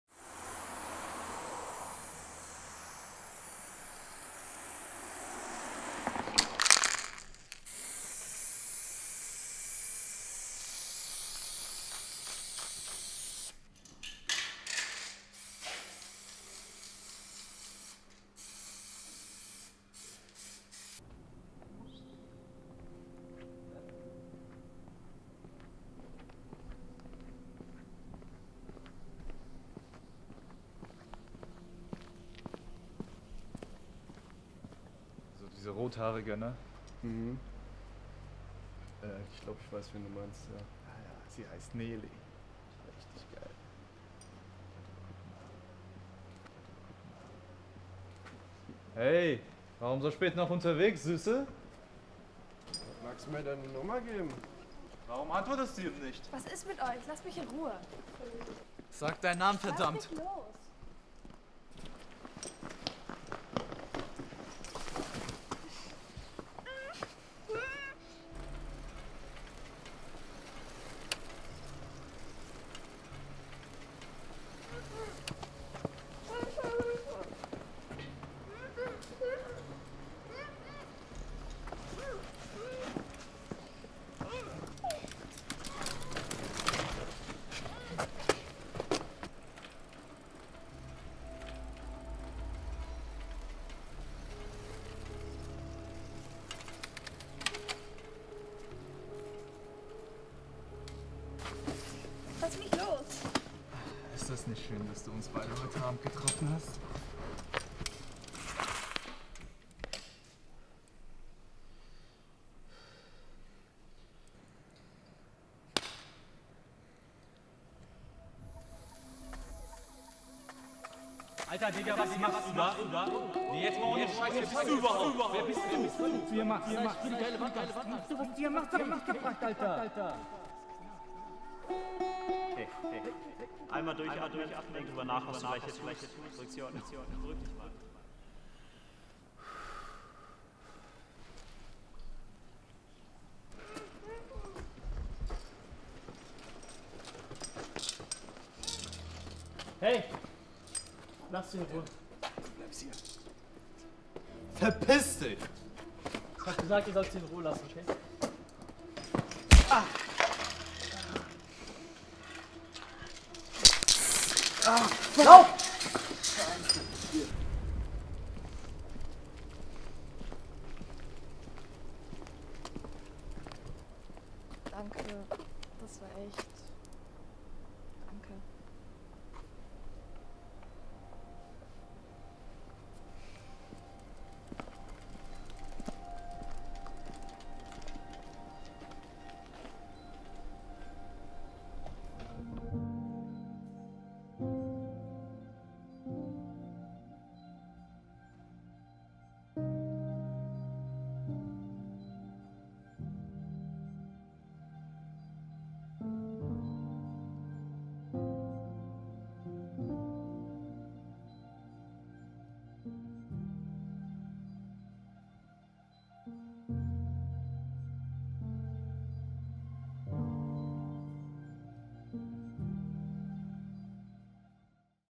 Heimweg | Kurzfilm
Unser Film handelt von einer jungen Frau, die nachts alleine auf dem Weg nach Hause ist. Weil sie von zwei jungen Männern belästigt wird, kommt unser Protagonist in die Situation, über seinen eigenen Schatten springen zu müssen, seinen ganzen Mut zusammen zu nehmen und die beiden zu konfrontieren.
Mit unserem Filmprojekt möchten wir unsere Zuschauer für das Thema Courage sensibilisieren. In der szenischen Darstellung werden die Folgen von unterlassener Zivilcourage verdeutlicht und an die Zuschauer appelliert, in schwierigen Situationen Mitgefühl und keinen Egoismus zu zeigen.
Regie:
Felix Schutt
Johannes Jensen

Besetzung:
Carla Schutt
Jakob Döhling
Erik Harder
Loarn Ippen
Mattis Strebel
Lasse Schafrinski

Crew:
Lilli Schutt
Max Wriedt

Weitere Unterstützung von:
Oskar Gallus
Finn Mangels
Karsten Baden Rühlmann
produced by Freshmen.Media